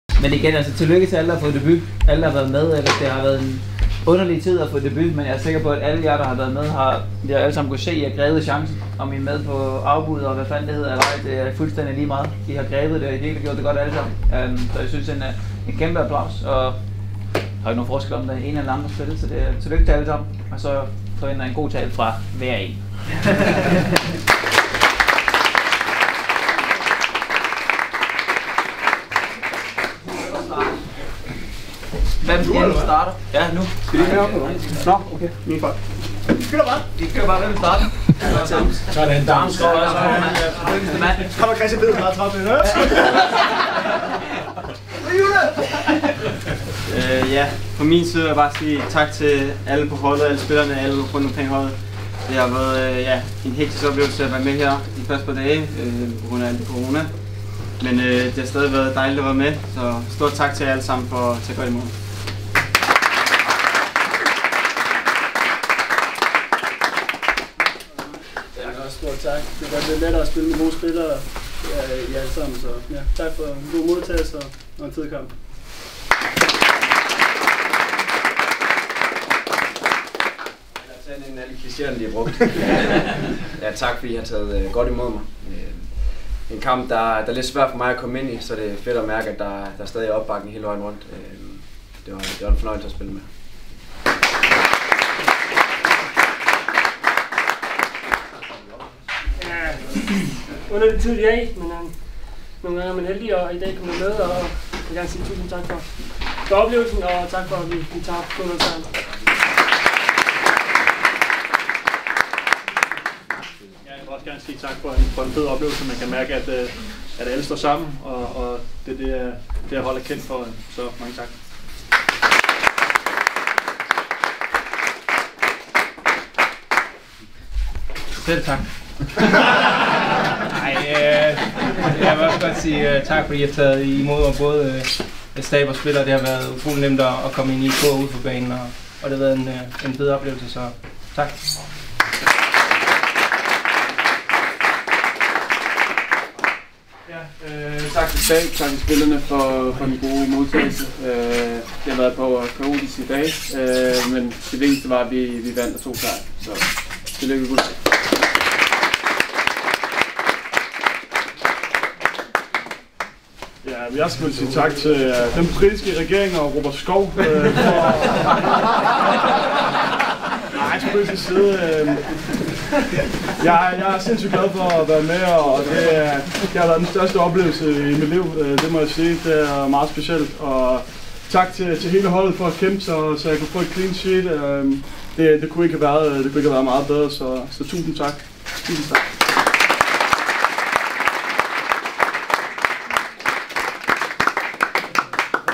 Landsholdet uden filter: Alle debuttaler efter sejren over Sverige
Christian Eriksen tog ordet i omklædningsrummet efter 2-0 sejren over Sverige og satte gang i en række debuttaler. En fast tradition som et rekordhøjt antal spillere måtte igennem efter landskampen.